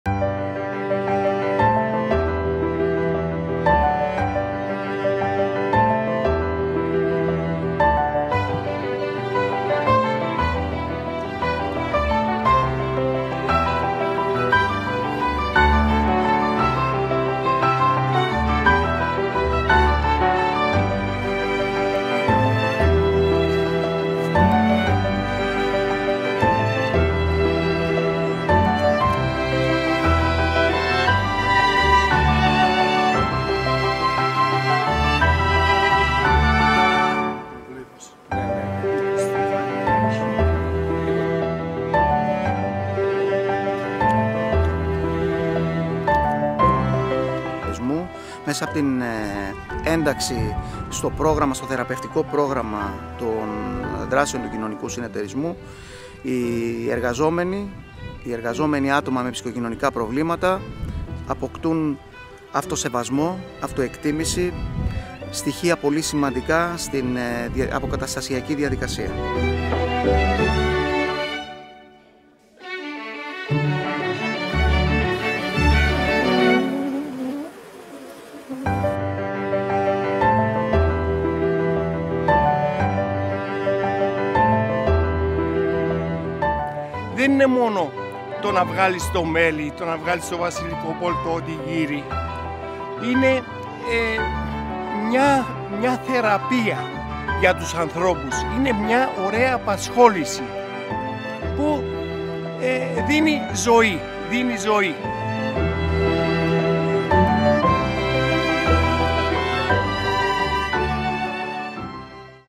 فائدة مدهشة للنحل.. كيف تستخدمه مستشفى يونانية مع المرضى؟
فائدة لا تُصدق.. تفاصيل استخدام مستشفى يونانية النحل في العلاج النفسي: "يساعد العقل على التركيز.. ويُعيد للمرضى ثقتهم"

ـــــــــــــــــــــــــــــــــــــــــــــــــــــــــــــــــــــــــــــــــــــــــــــــــــــــــــــــــــــــــــــــــــــ
تيلغرام
سناب شات alarabiya
تفاعلكم
صباح العربية
بانوراما
العربية الليلة
Live Alarabiya
Alarabiya Live